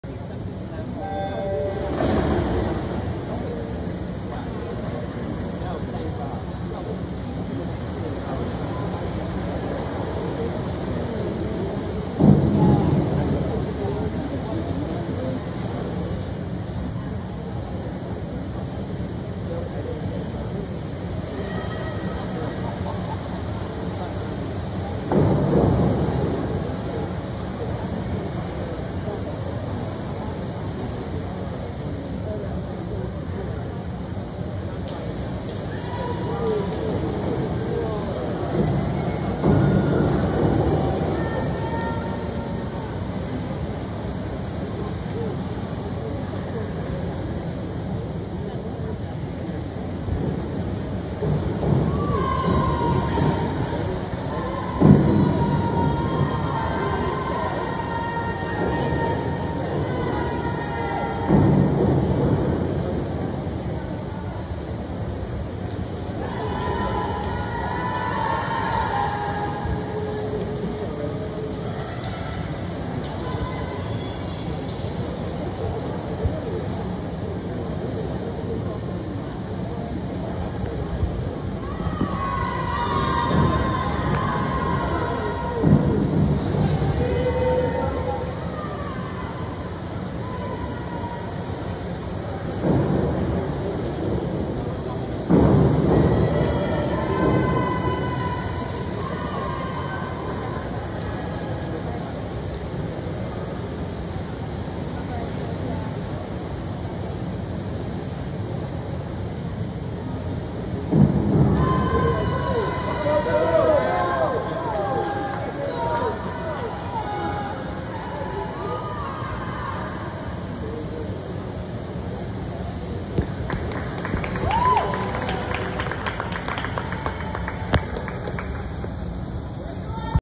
2024 Women’s Dual vs Houston, 200 Backstroke, Heat 1
Lane 2: Gracie Walker
Lane 4: Aviv Barzelay
Lane 6: Abigail Ahrens
Lane 8: Alice Marini